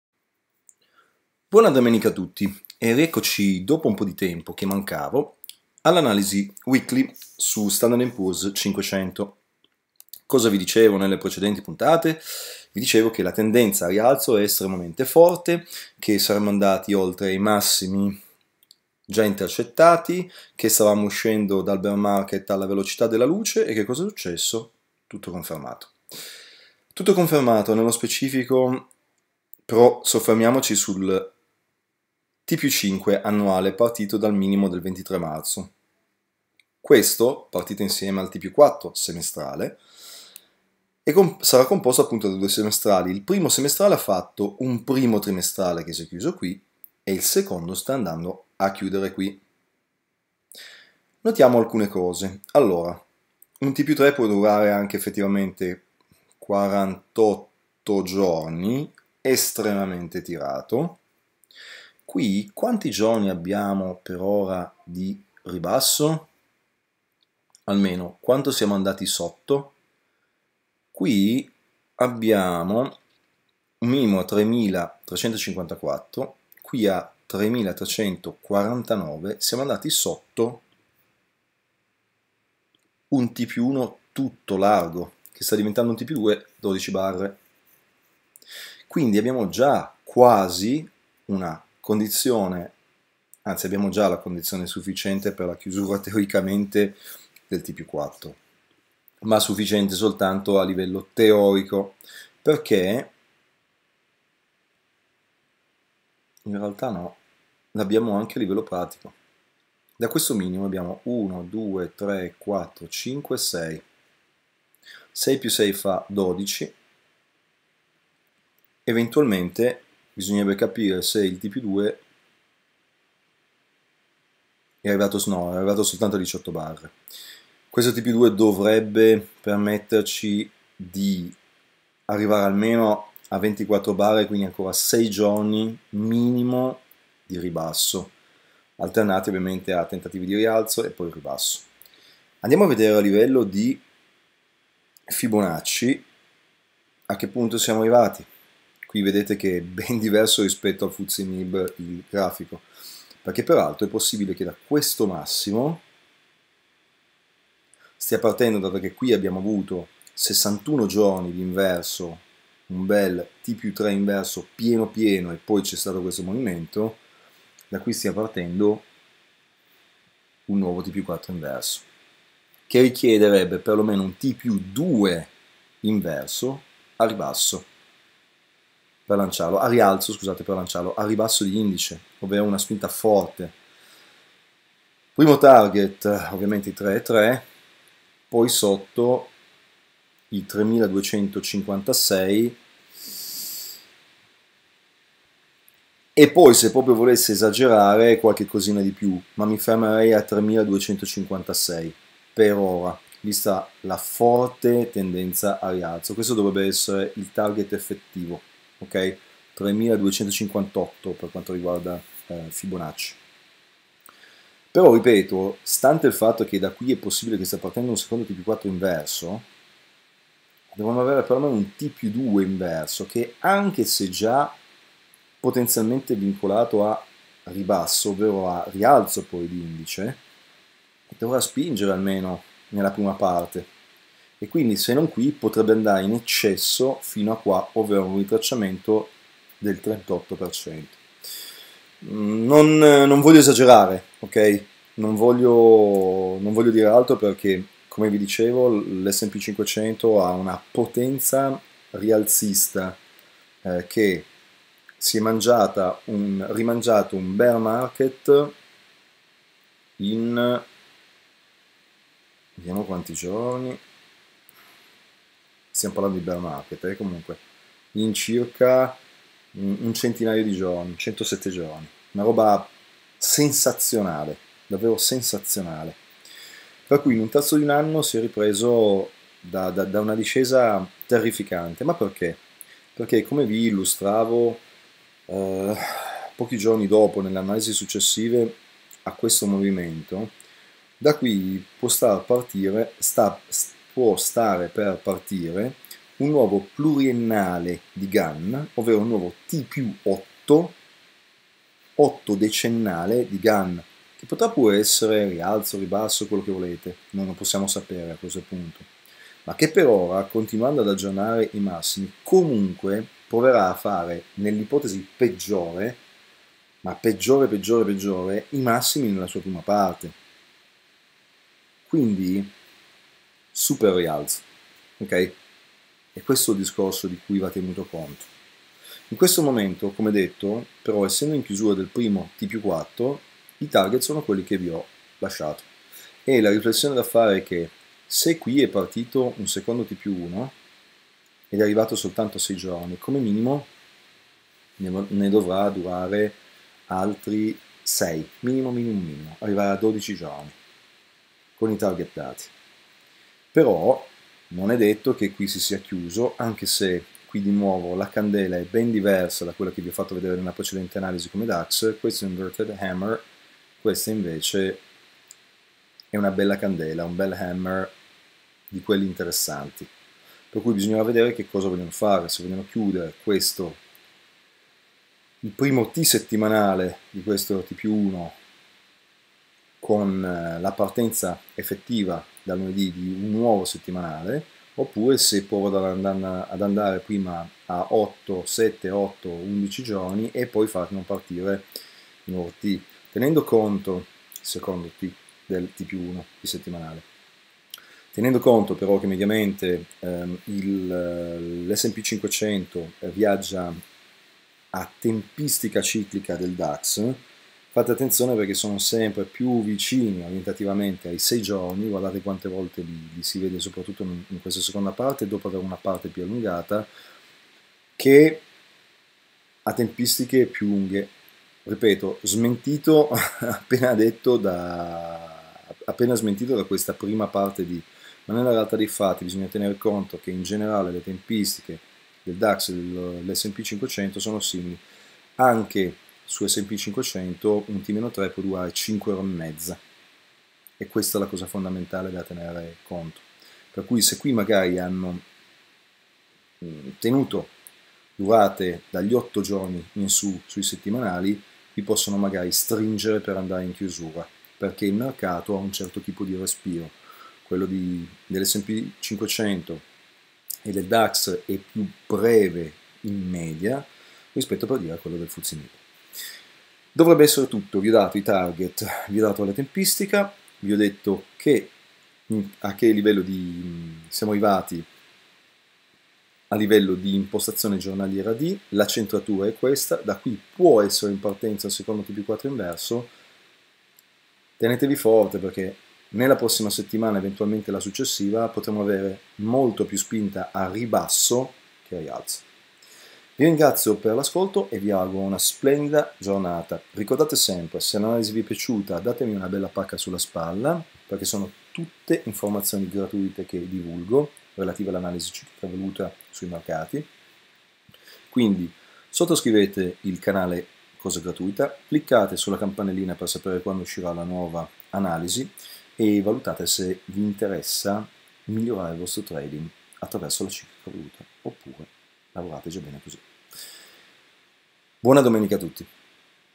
S&P500 - Analisi Evoluta 22-2020 - Chiusura di T+4 Semestrale e prosecuzione della discesa
Buon giorno,

Nella video analisi i target ed il tempo ciclico per la chiusura del T+4 Semestrale e la partenza di qualcosa di interessante di inverso. Privilegiate operazioni short sui top con tecniche di money management, se conoscete molto bene l'Analisi Ciclica Evoluta.

Buona prosecuzione.

Jacopo